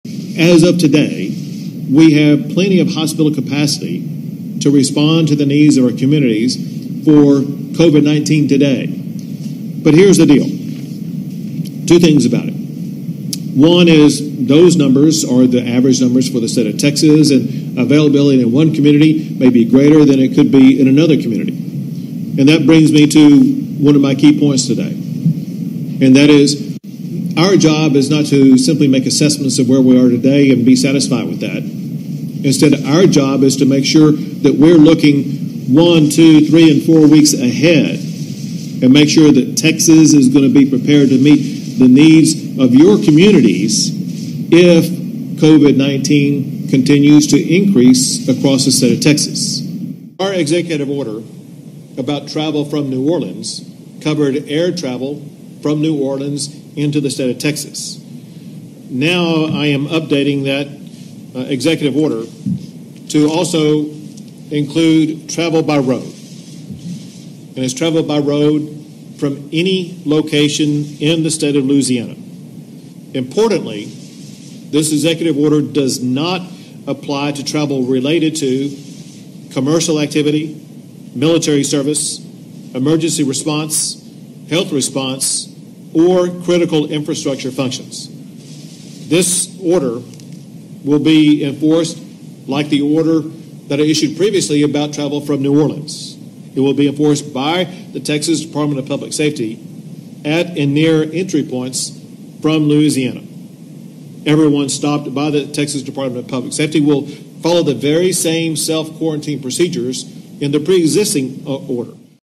Texas Governor Orders DPS Troopers To Stop Drivers Coming Out Of Louisiana And Quarantine Them
Texas Governor Gregory Abbott expanded an executive order Sunday that requires people traveling into Texas by road from Louisiana to self-quarantine.

Previously, Texas was requiring anyone flying in from New Orleans to be quarantined for 14 days while they stay in Texas.

Sunday, the order was expanded to include anyone traveling from anywhere in Louisiana into Texas.

Gov. Abbott said: "My prior executive order about travel from New Orleans covered air travel from New Orleans into the state of Texas. Now, I am updating that executive order to also include travel by road. Travel by road from any location in the state of Louisiana. Importantly, this executive order does not apply to travel related to commercial activity, military service, emergency response, health response, or critical infrastructure functions. This order will be enforced like the order that I issued previously about travel from New Orleans. It will be enforced by the Department of Public Safety at and near entry points from Louisiana."

The flight order also included New York, New Jersey and Connecticut to prevent the spread of COVID-19.

The quarantine will be executed by the Texas Department of Public Safety.

For people on airlines, Gov. Abbott said that individuals flying from these specific destinations will have to fill out a form by the Texas Department of Public Safety that will designate where the individual will quarantine.

DPS troopers will visit the individuals to verify that they are following the executive order. Anyone who does not comply with this order will be subject up to a $1,000 fine, 180 days in jail or both.